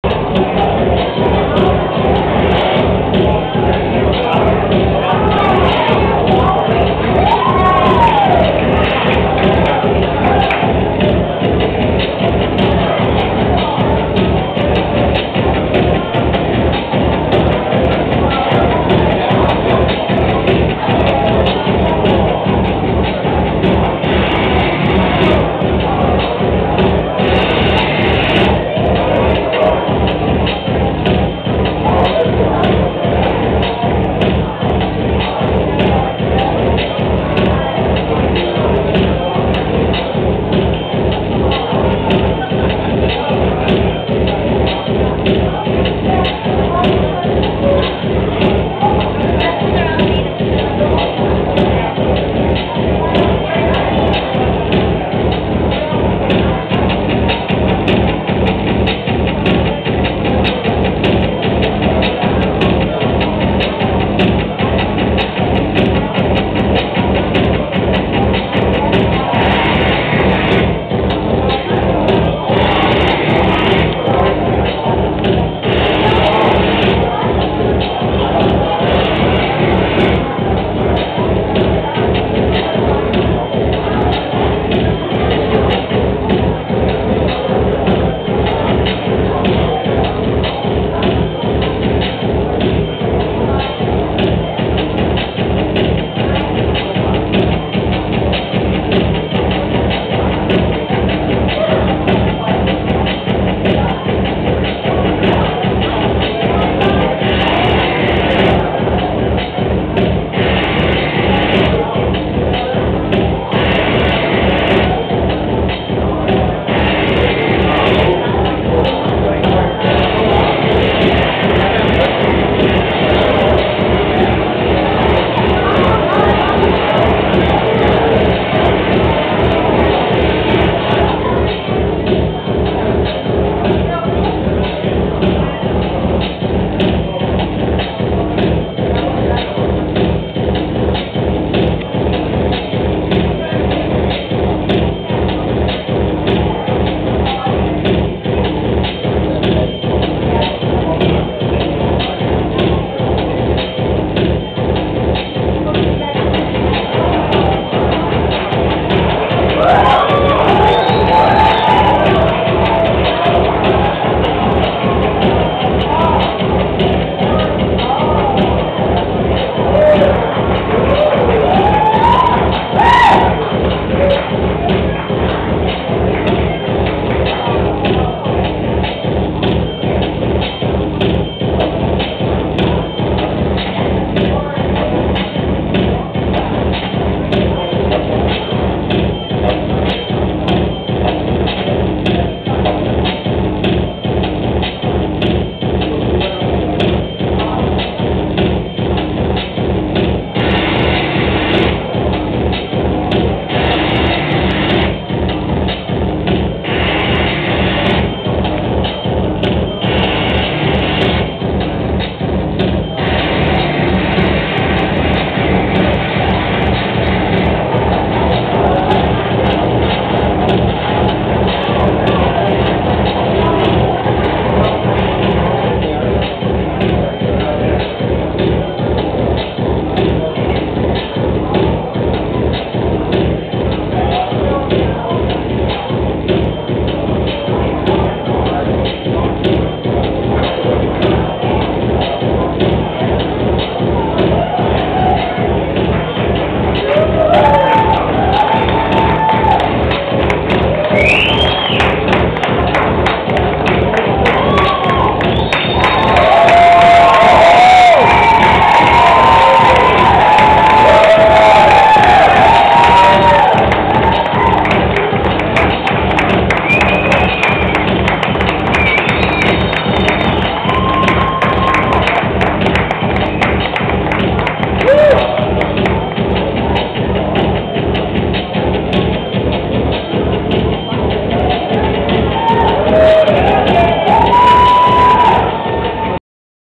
2011 DAM Design Show - Cristin Richard
The 2011 DAM Design Show opened with a preview party featuring a runway show.  This year's theme is "BATH", and the designers showed their vision for bath, spa, and loungewear.  This clip features work by Cristin Richard.